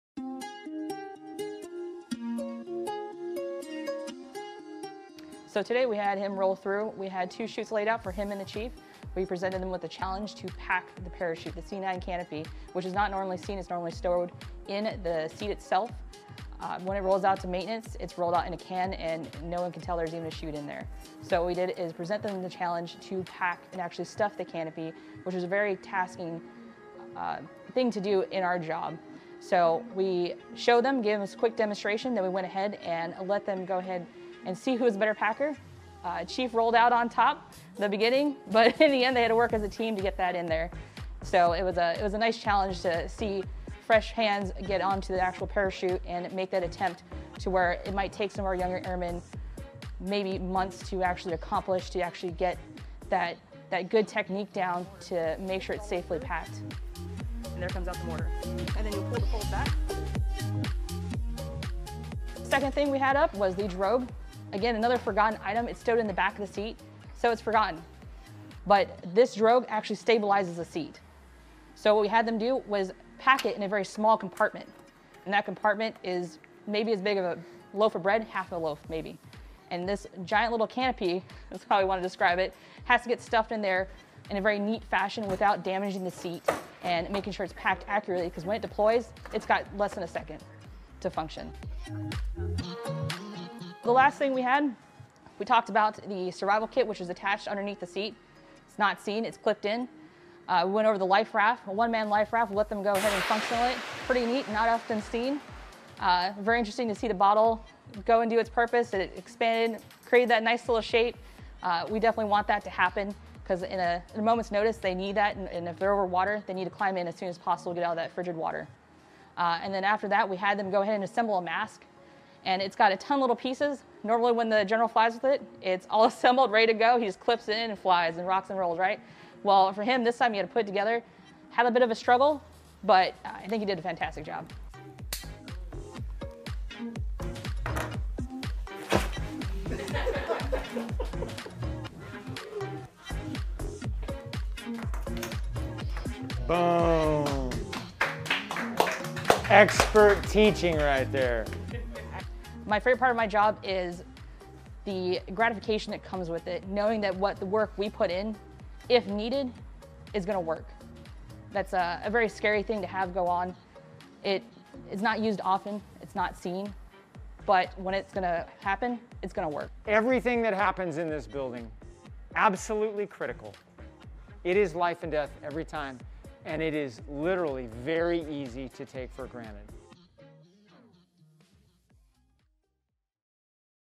31st OSS Aircrew Flight Equipment team to learn how that make the mission happen everyday!
RUYS: 31st OSS AFE
ITALY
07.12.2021

For this week's Roll Up Your Sleeves, we visited the 31st OSS Aircrew Flight Equipment team to learn how that make the mission happen everyday! Let's take a look!

Film Credits: Video by Senior Airman Ericka Woolever
31st Fighter Wing Public Affairs